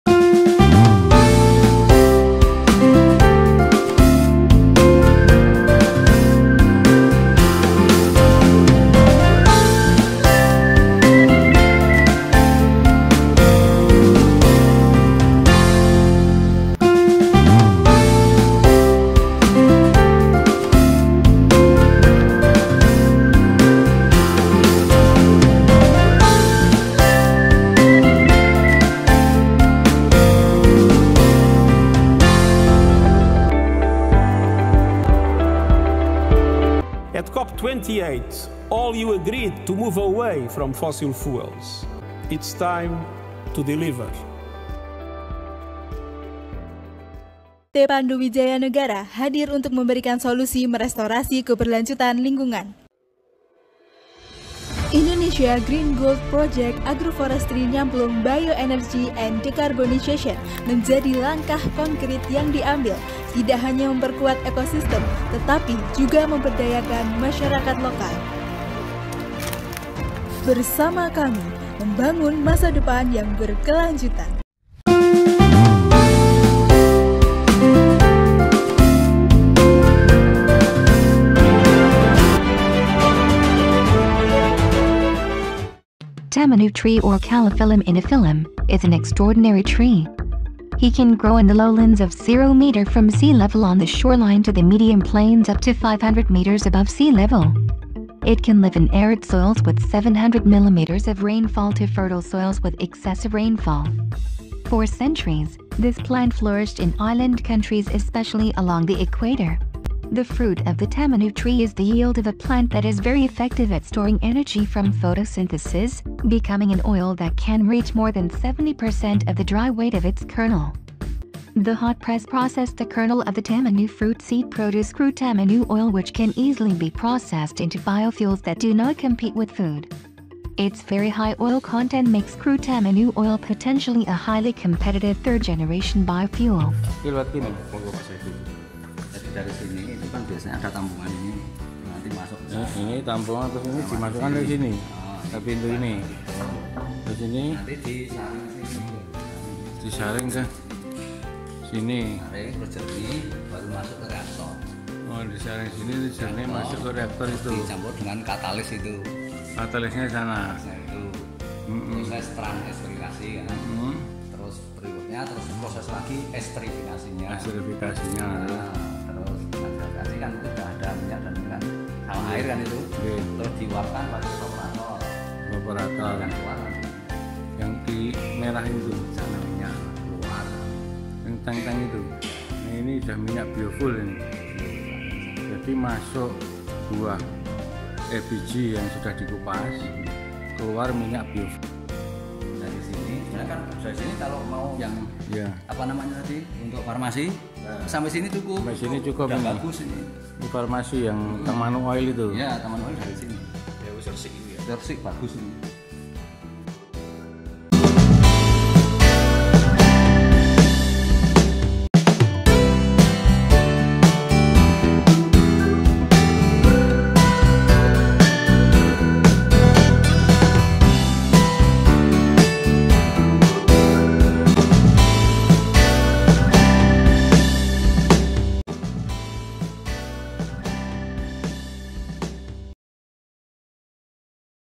Pohon nyamplung Tanaman hutan penghasil bioenergi Serapan karbon dpt diperdagangkan di bursa karbon
PIC:  Edi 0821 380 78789